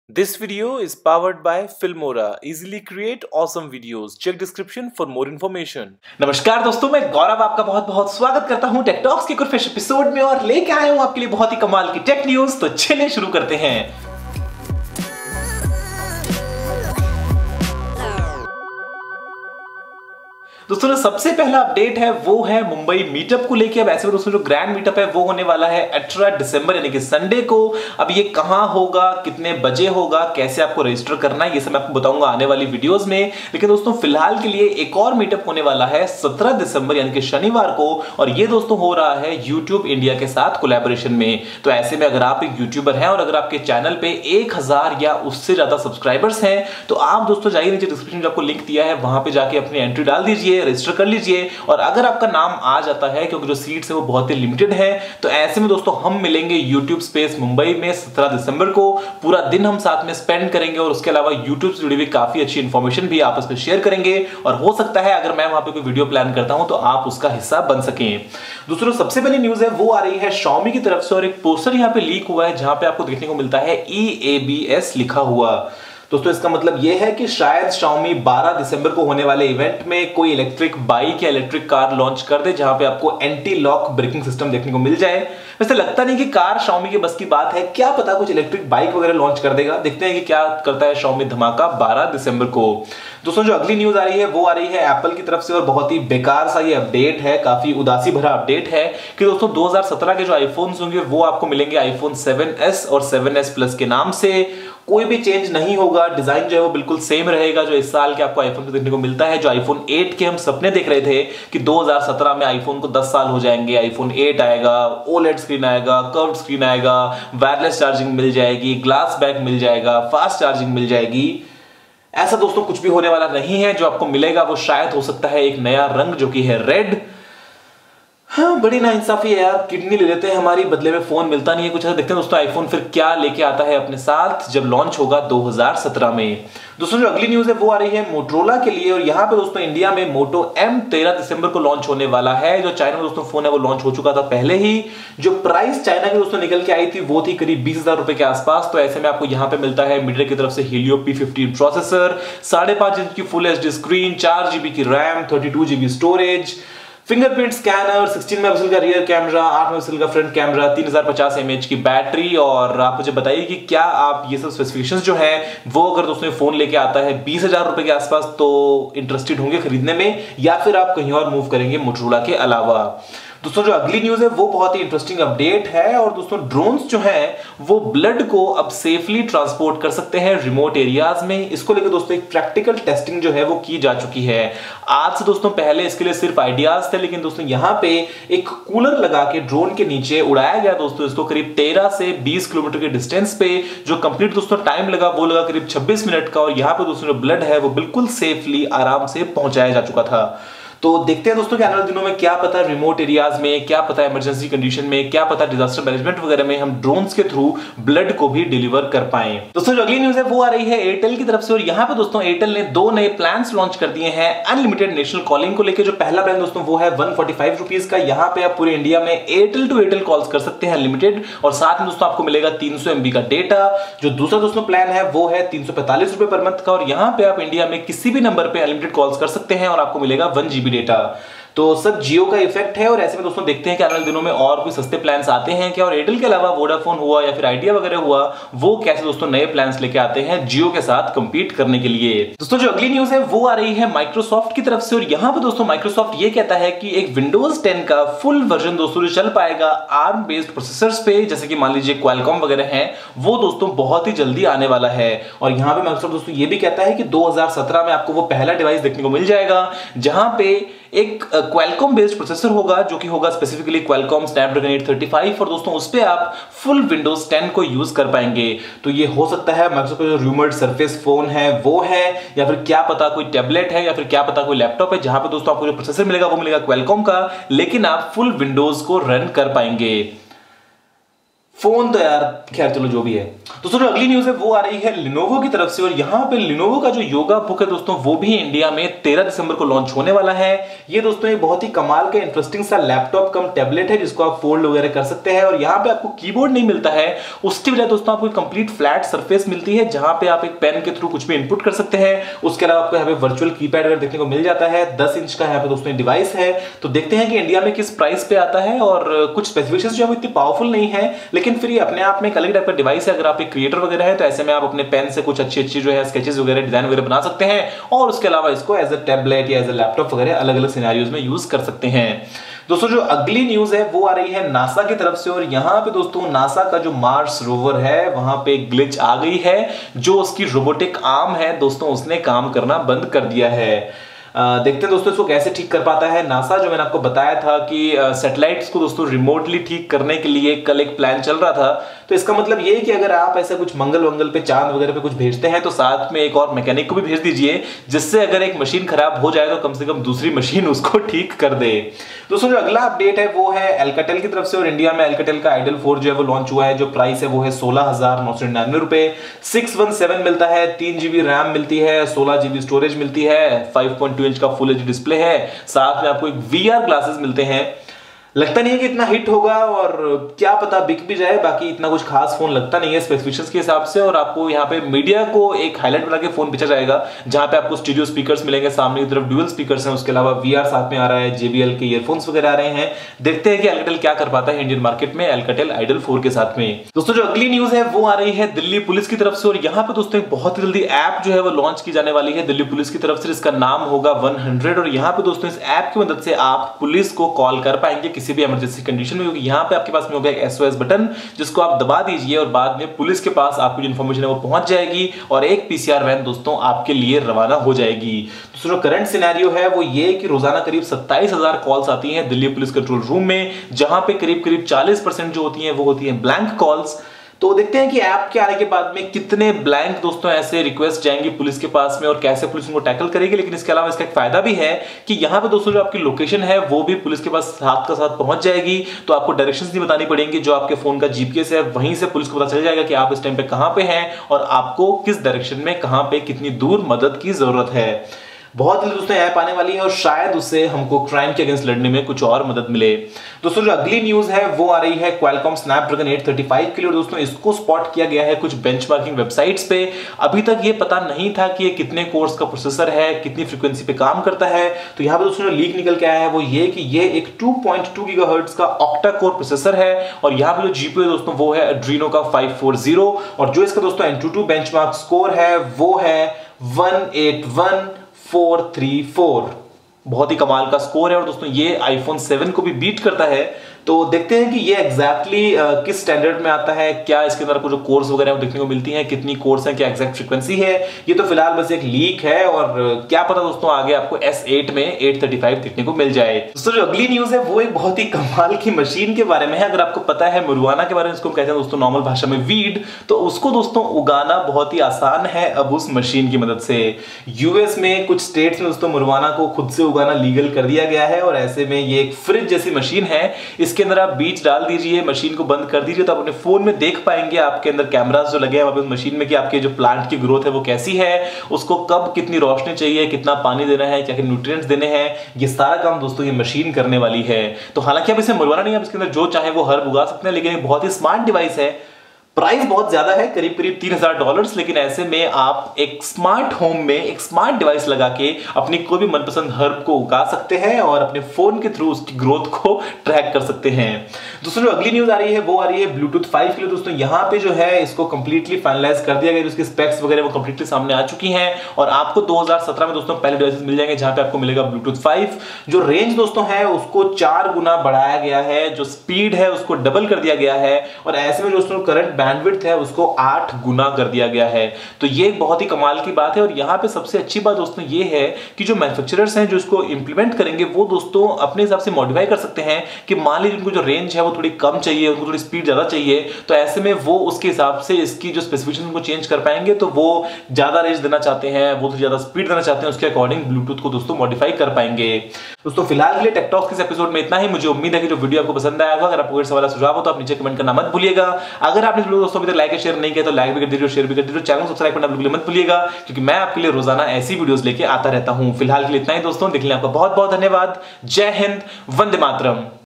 Tech Talks #61 - Mumbai Meet Up, Xiaomi Car, Marijuana at Home, iPhone 7S, Drone for Blood
Namaskaar Dosto, Tech Talks ke is Episode mein maine aapse bahut si interesting Tech News Share ki hai.

Apple iPhone 7s, Xiaomi Electric Car, Mumbai Meet up Details, Marijuana At home, NASA Mars Rover, Airtel Unlimited plans aur bahut kuch. Mujhe umeed hai ki yeh video aapko pasand aayega.